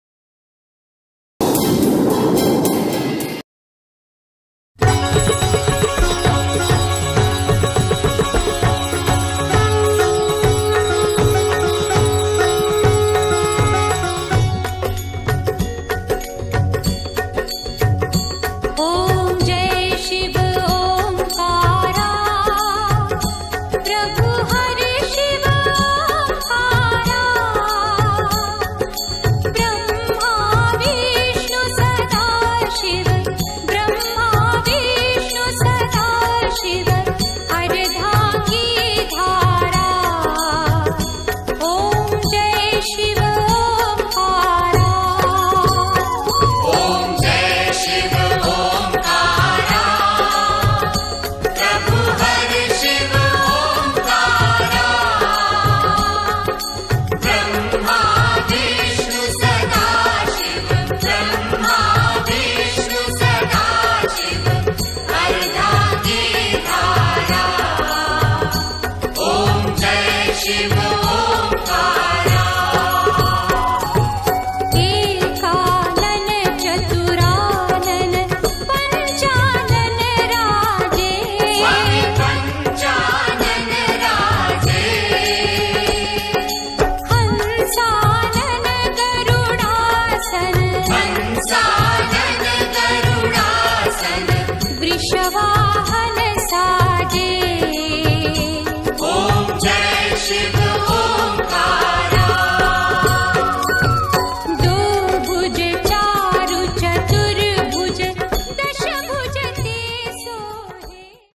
जयपुर : नाहर सिंह बाबा मंदिर में महादेव की आकर्षक बर्फ की रंगीन झांकी का आंनद ले।
रिपोट : एस.एन.विवेक।